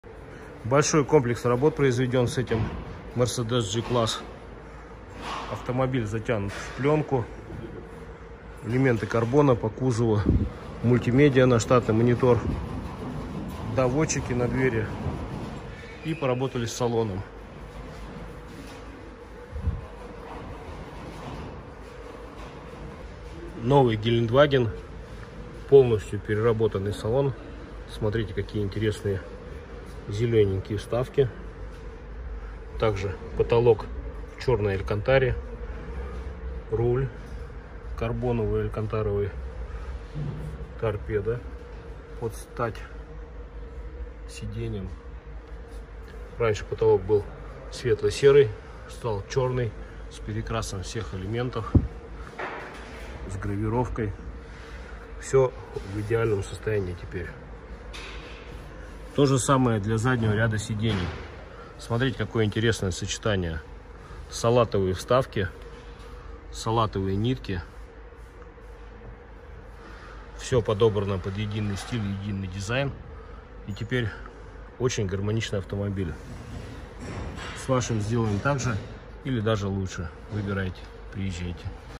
G-class - КОМПЛЕКСНОЕ ДООСНАЩЕНИЕ
Полностью проработали Mercedes G-class!!!

Большой комплекс работ произведен с этим Mercedes G-class. Автомобиль затянут в пленку, элементы карбона по кузову, мультимедиа на штатный монитор, доводчики на двери и поработали с салоном.